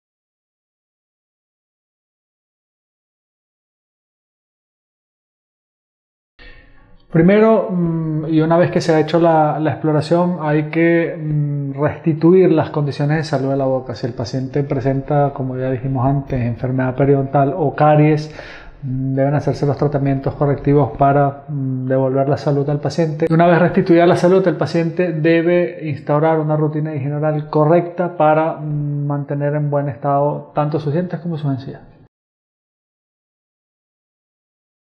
¿Cómo se puede prevenir que la halitosis vuelva a aparecer?
El Dr. Walter Michael Stumpf, director médico de Centros de Calidad Dental,  nos explica como prevenir la halitosis.